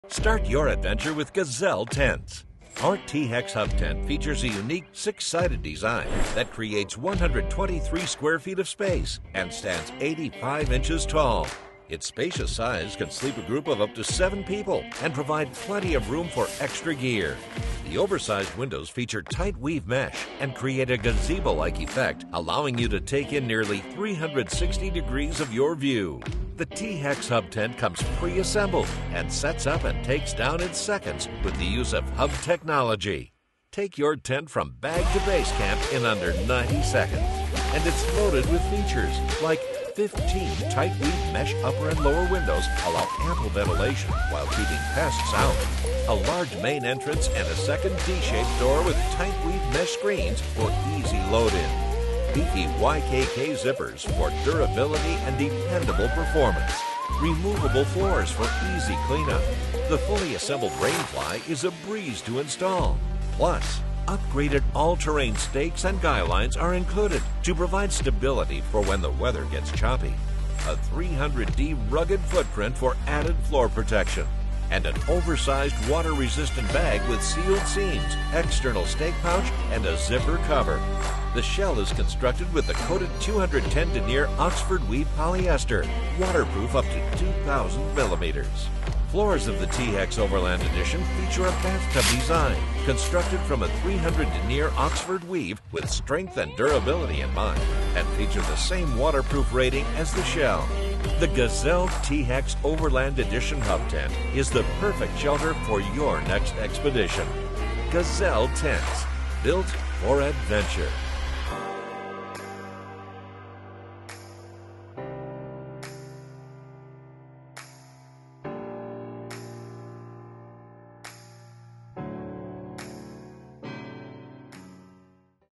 Gazelle Tents T-Hex Hub Tent
Gazelle Tents™ rugged design and ease of set up makes the perfect ground tent for your next overland or car camping excursion. The T-Hex Hub Tent Overland Edition is a unique 6-sided designed tent that creates 123 square feet of space and stands nearly 7’ tall. Its spacious design can sleep a group of up to seven people with plenty of room for extra gear. The oversized windows create a gazebo like effect allowing you to take in nearly 360-degrees of your view. Each window features TriTech Mesh—a single filament mesh that has increased durability and improved airflow. Take your T-Hex from bag to base camp in under 90 seconds. It comes ready to use, no assembly required and no loose parts, using our hub technology.